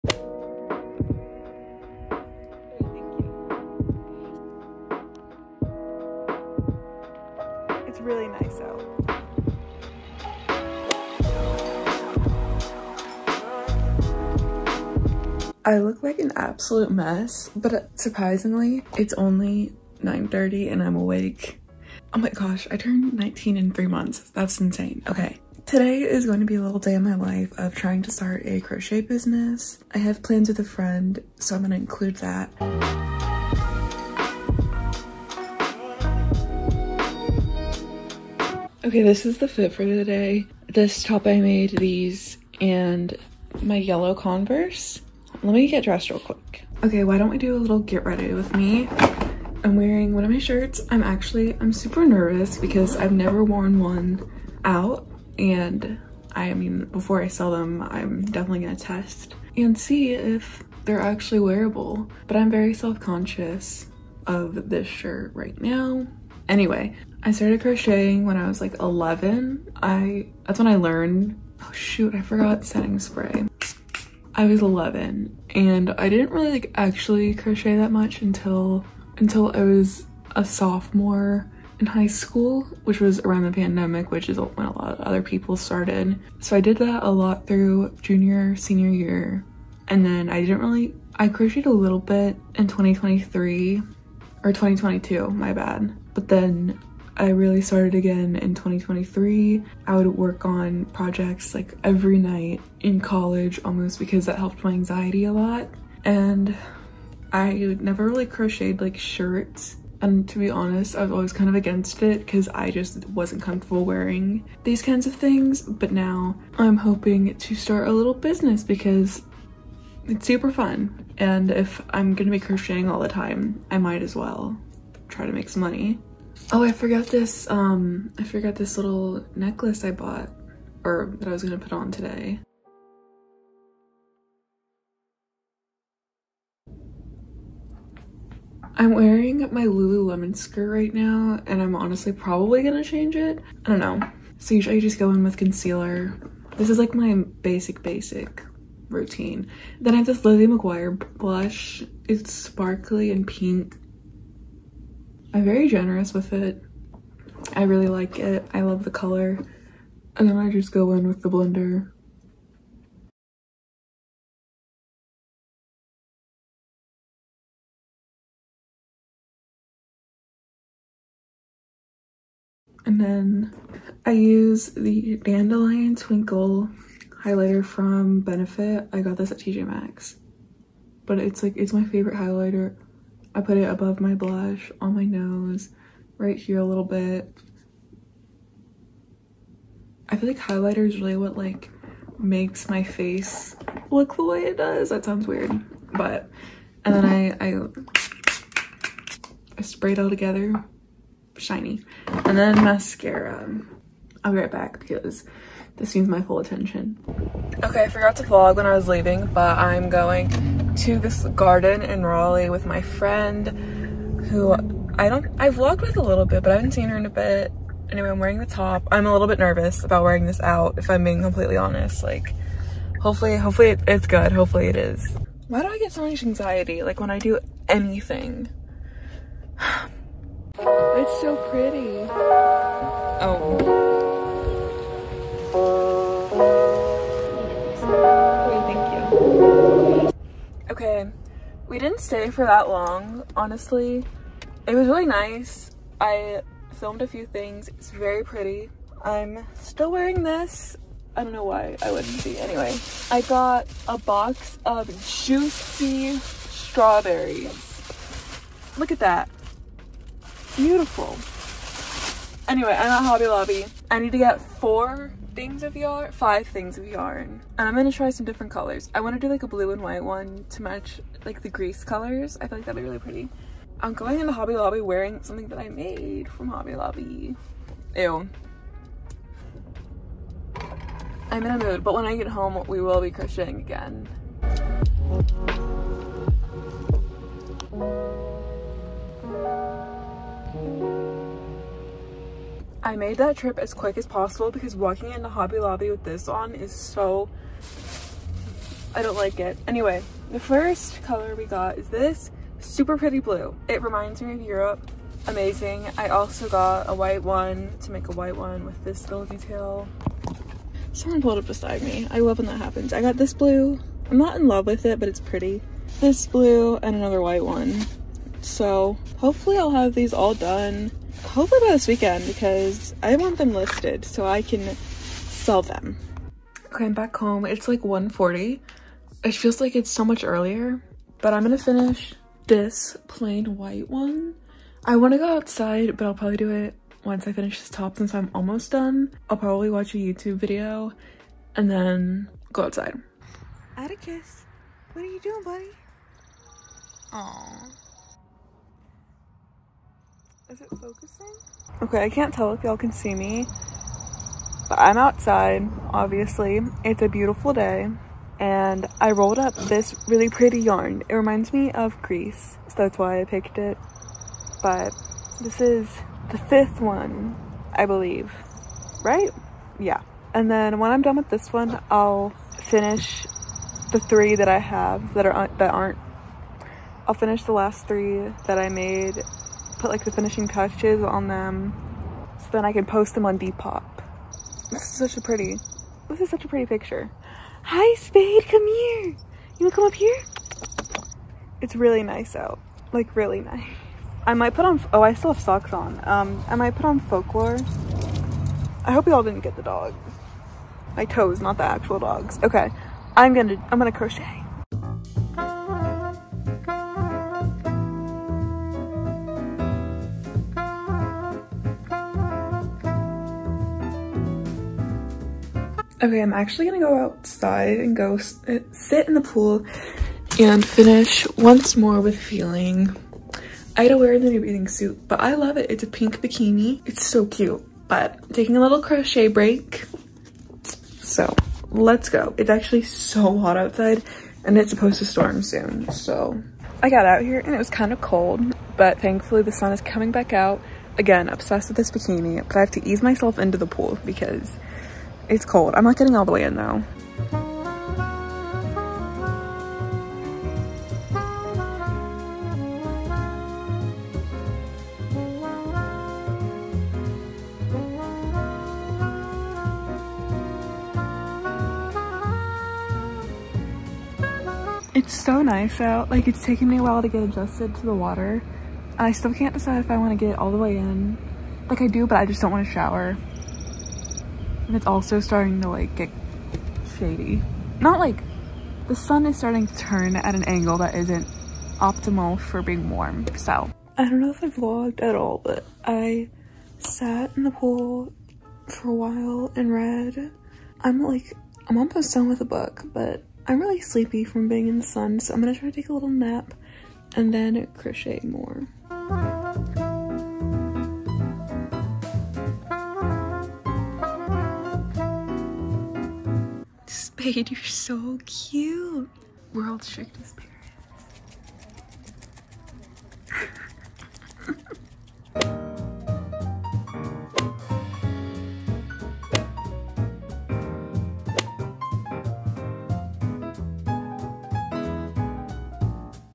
my crochet business: a day in my life starting a crochet business on etsy
i'm starting a crochet business! i've been crocheting a lot in the past few years, and i'm finally making the crochet business i've always wanting to start. i learned how to make some really cute crochet tops on tiktok, and i want to sell them. i hope you guys enjoy, and if anyone also likes to crochet or has a business, feel free to dm me!

follow me on:

Crochet TikTok- @caro.crocheted
Pintrest- @carolineeelizabeth04
Depop- @carolineeelizabeth04